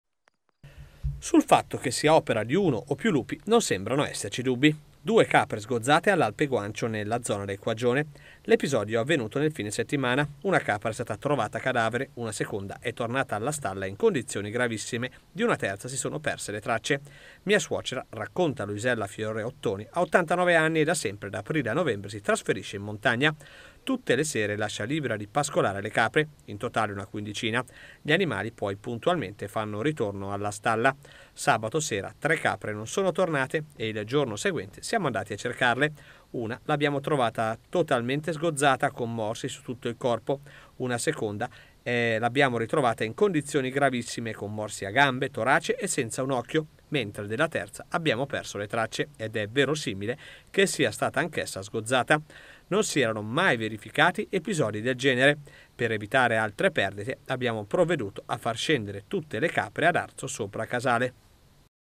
Capre sgozzate in valle Strona probabilmente dai lup
15 ottobre 2019 - E' accaduto nel fine settimana all'alpe Guancio, nella zona del Quaggione